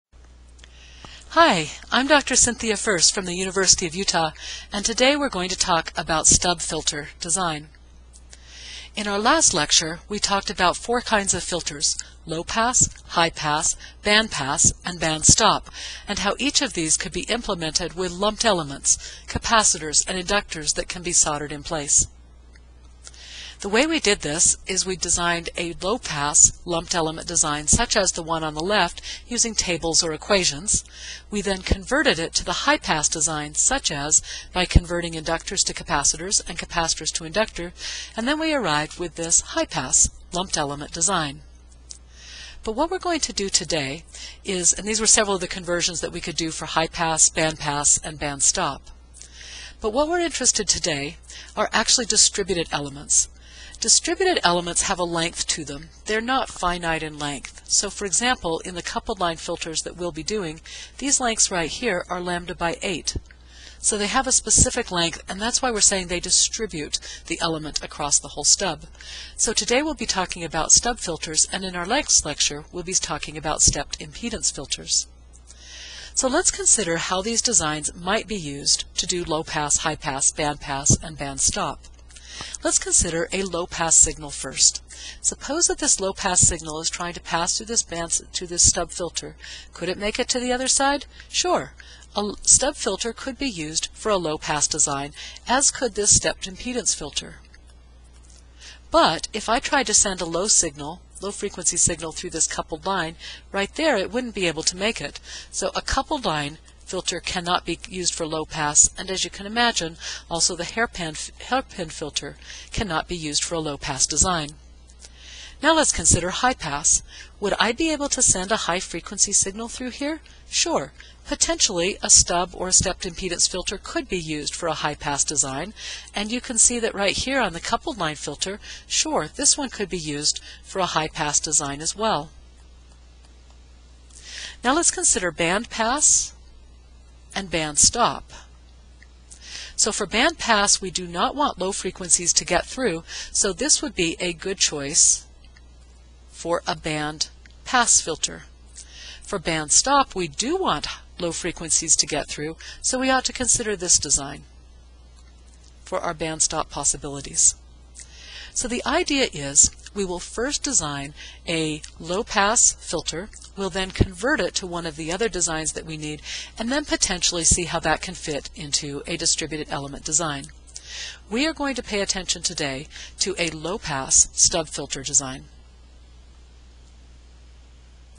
Microstrip Stub Filters -- Part 1
Introduction to design of microstrip stub filters, quick intro without the math.